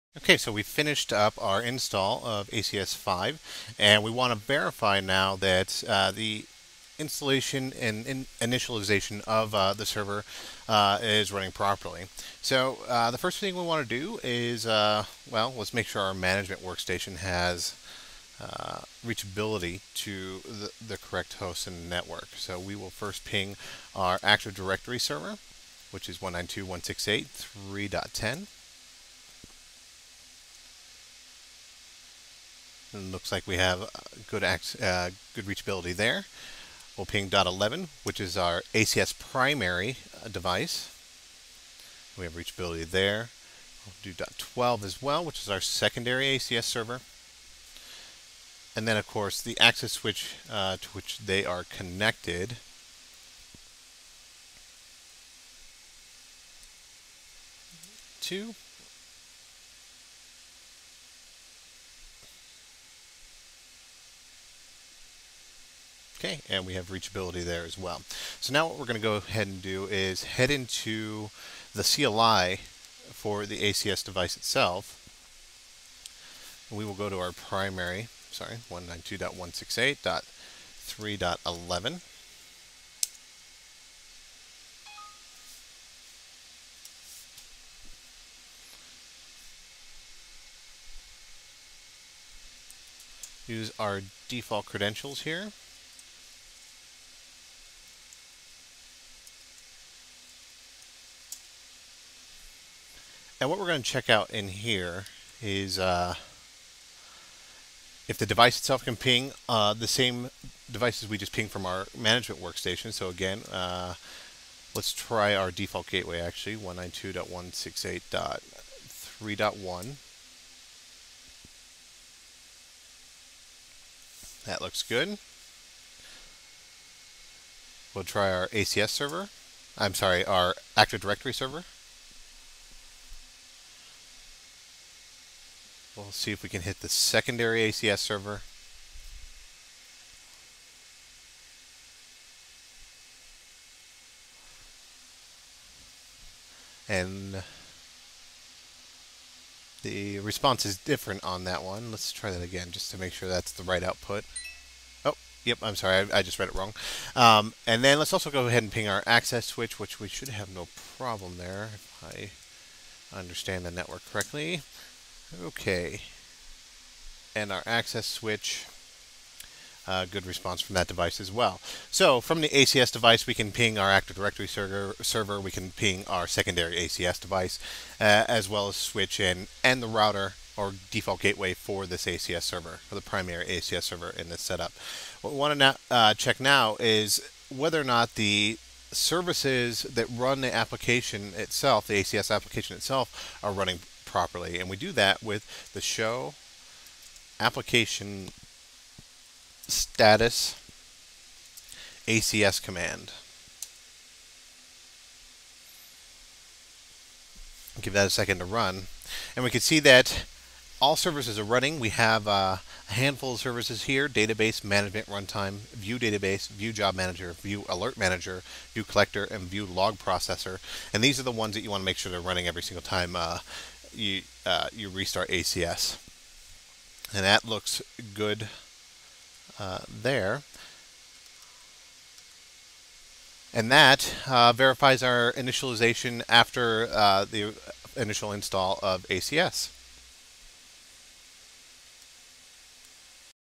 ACS Verification
The steps following ACS 5 installation that verify the proper operation and reachability of the ACS server.